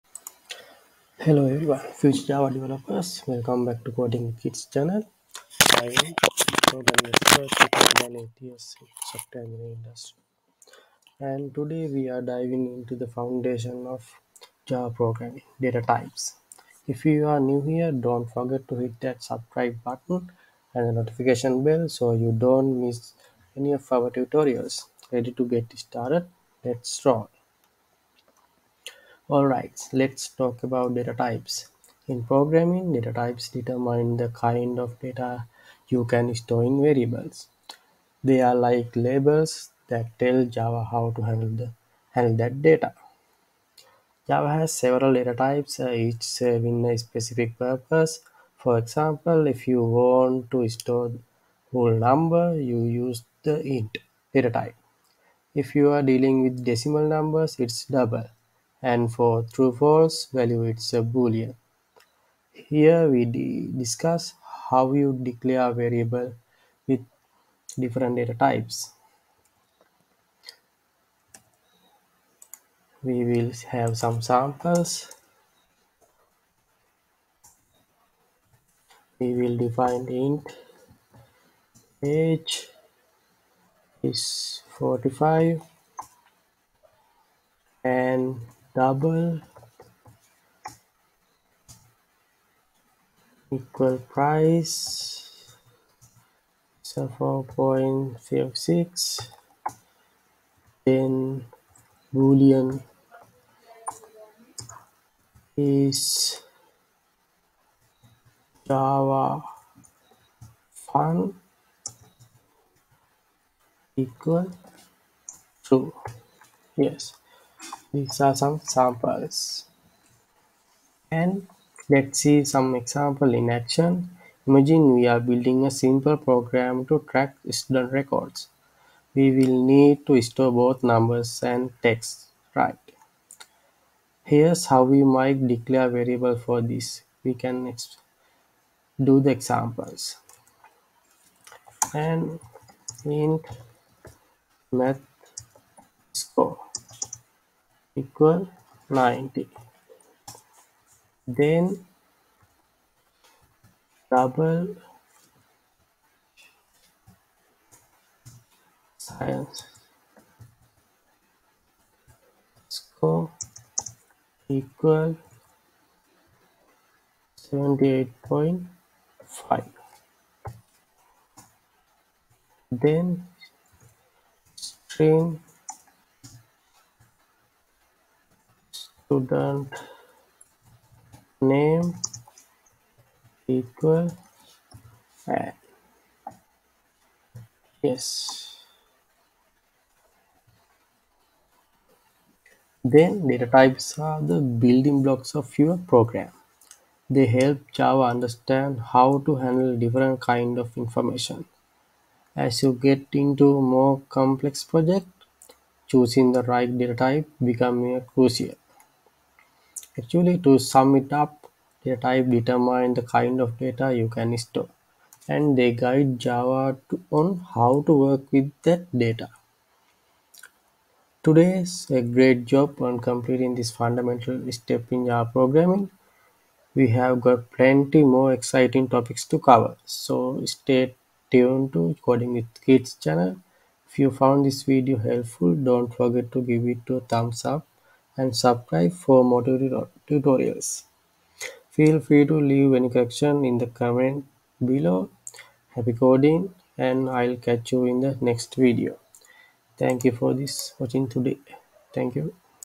Java Programming Basics: Understanding Data Types | Step-by-Step Tutorial
Discover the essentials of Java programming with our comprehensive guide, "Java Programming for Beginners: Understanding Data Types." Dive into the world of data types and their crucial role in programming. This beginner-friendly tutorial provides a clear understanding of Java's data types, making it an ideal starting point for those new to programming. Elevate your coding skills and embark on a successful programming journey with us!